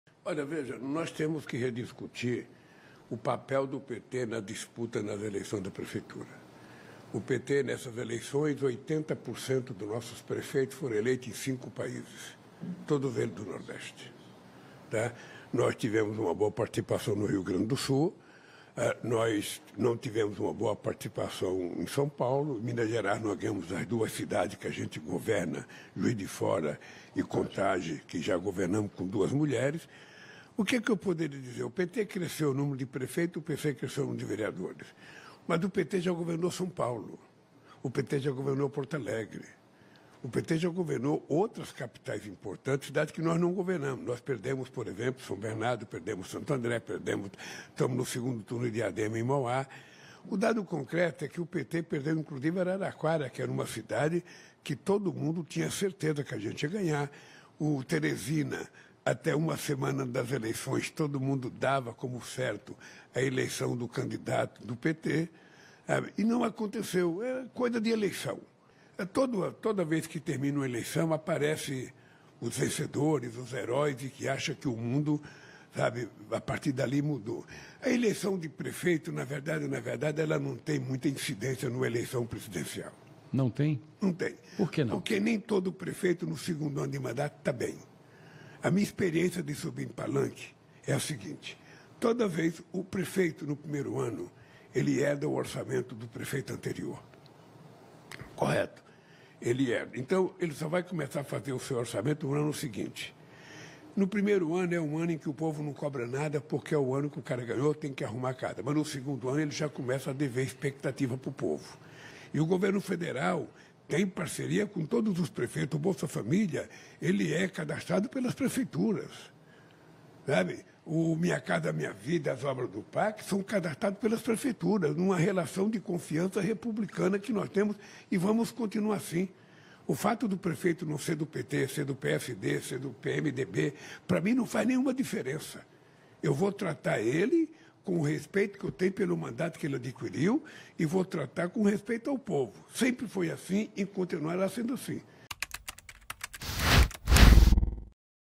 "Temos que rediscutir o papel do PT nas eleições para prefeituras", afirma Lula
O presidente Luiz Inácio Lula da Silva disse, nesta sexta-feira, 11, que é preciso "rediscutir o papel do PT na disputa pelas prefeituras". O petista fez uma autocrítica sobre o desempenho do partido e citou que "não tivemos (o PT) uma boa participação em São Paulo (o Estado)", por exemplo. DETALHES